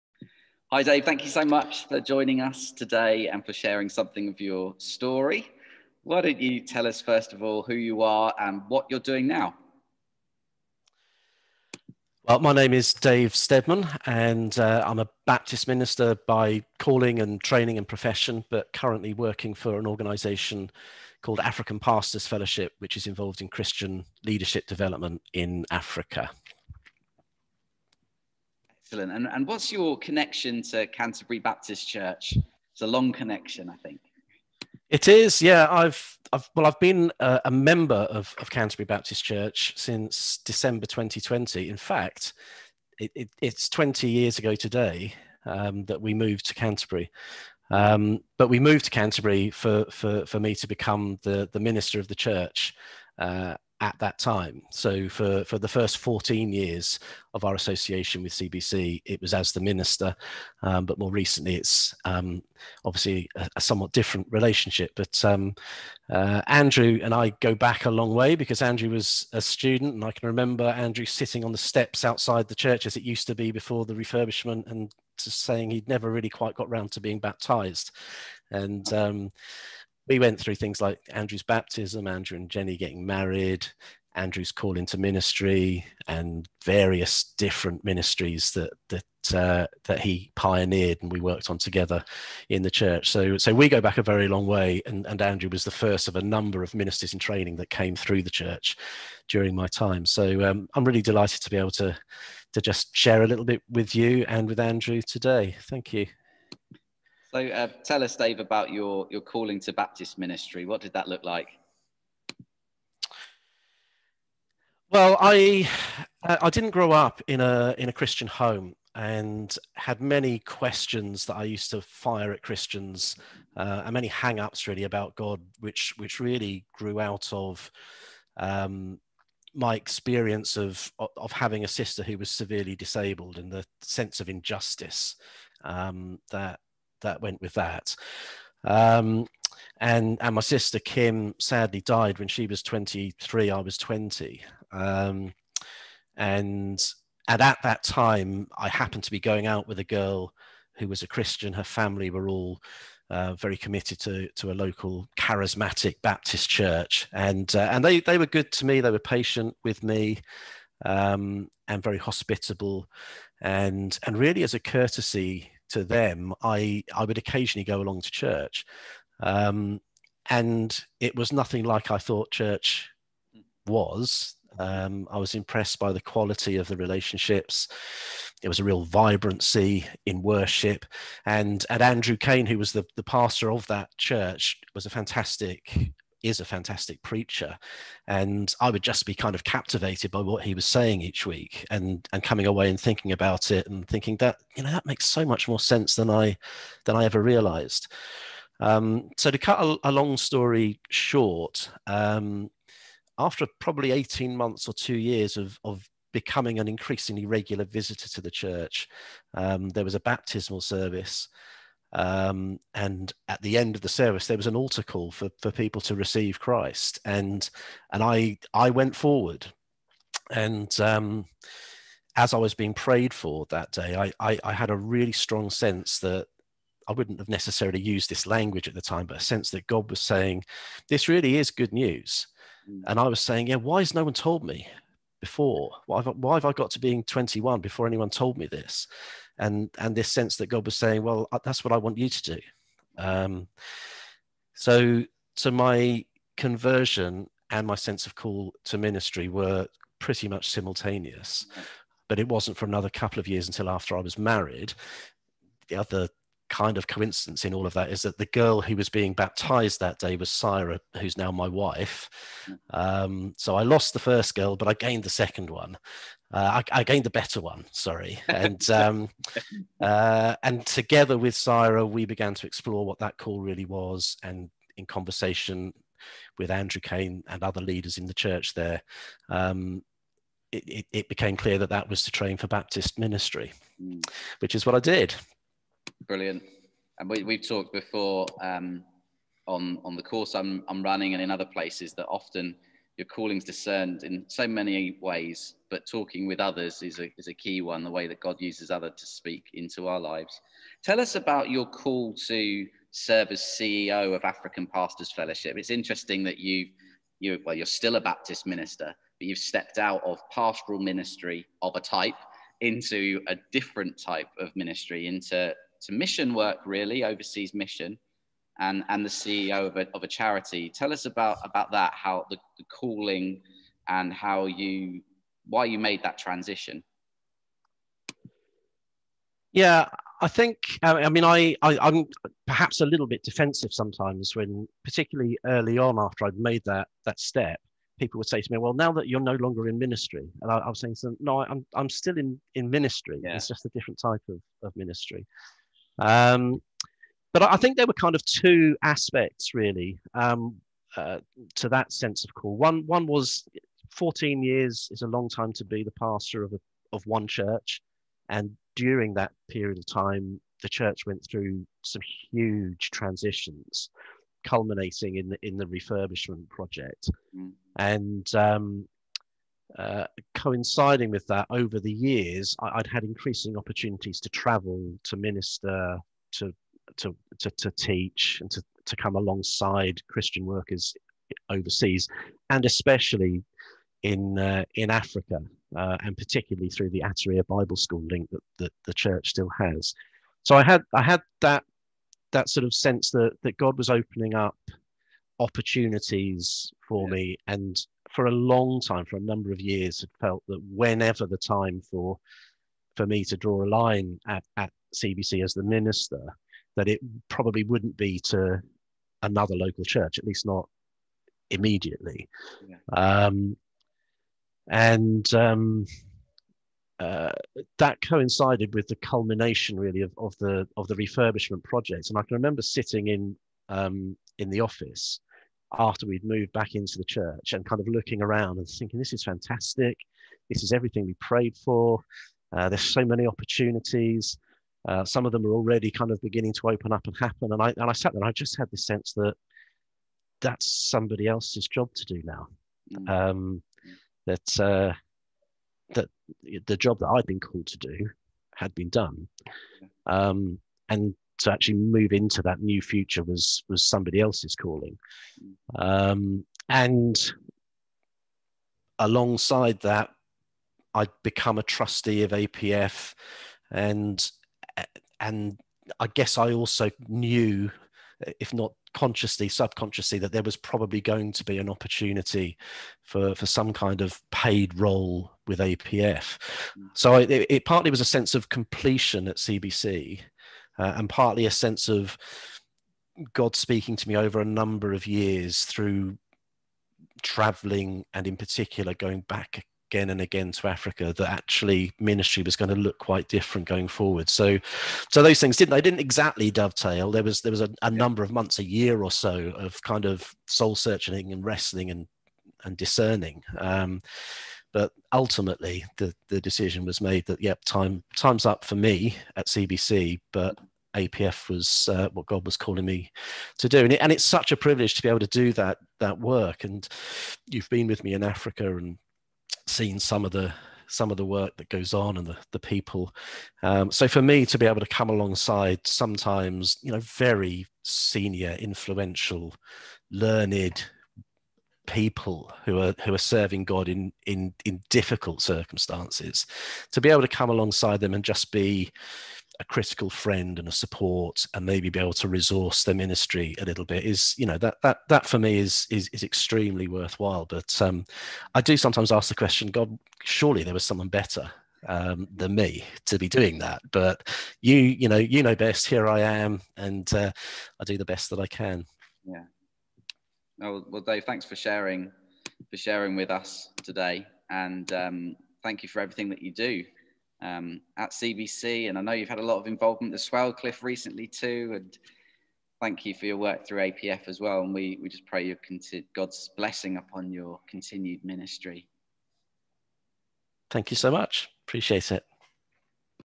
The 'Sent' Interviews with Rev Dave
Formally minister of the church, Dave now serves as CEO of African Pastors Fellowship